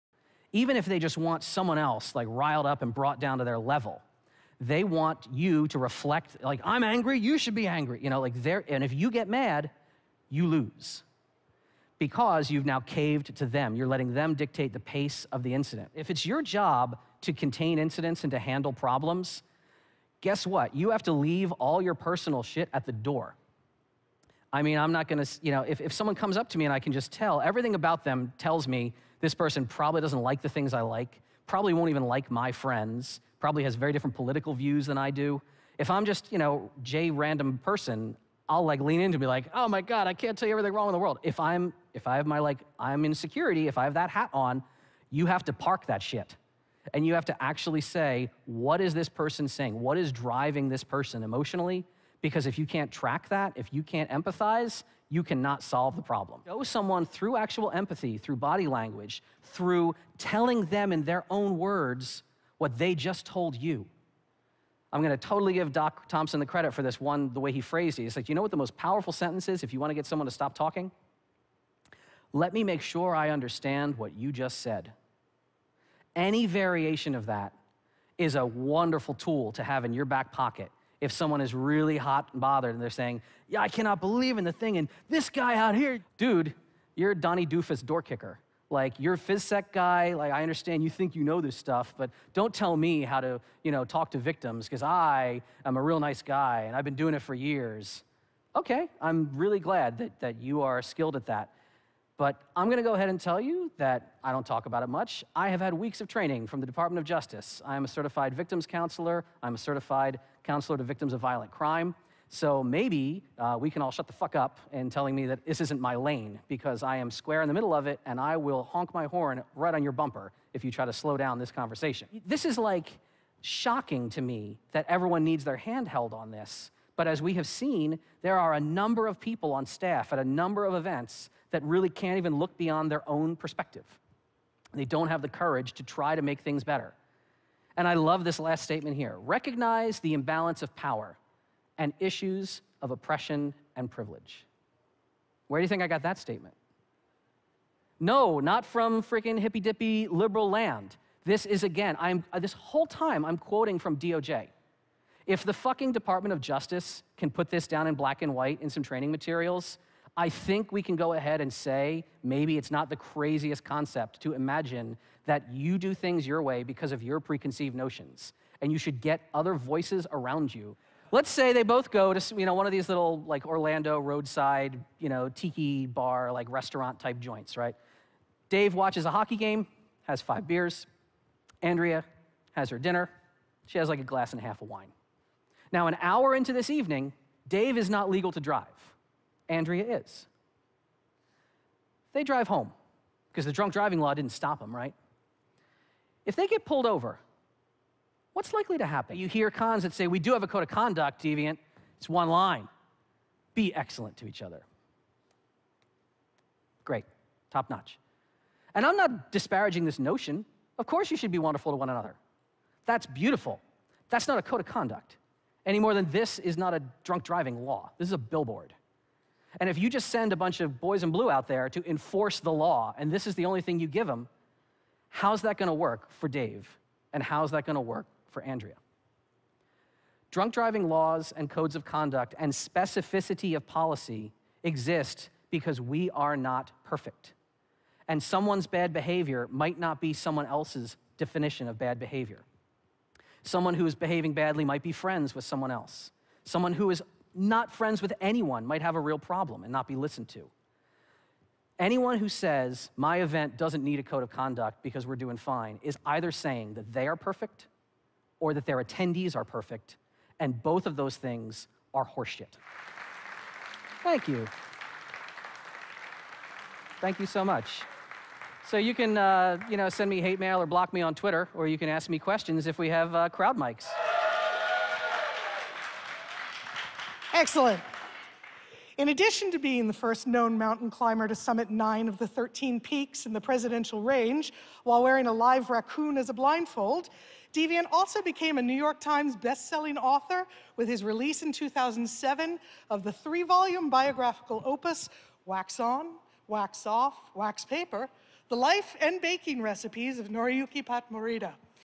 BSides Orlando "The Silver Tongue vs The Iron Fist" talk preview
My talk at BSides Orlando 2019 was all about running physical security operations for conferences and events.  In an arena where words are your most powerful weapons and you most useful tools, I ask all in attendance to consider what training and techniques they've learned and practiced when it comes to conflict de-escalation, crisis management, and victim counseling.

The full talk is up on their YouTube channel... ...and I'd value and appreciate your thoughts and feedback if you choose to check it out.

Best regards and stay safe out there!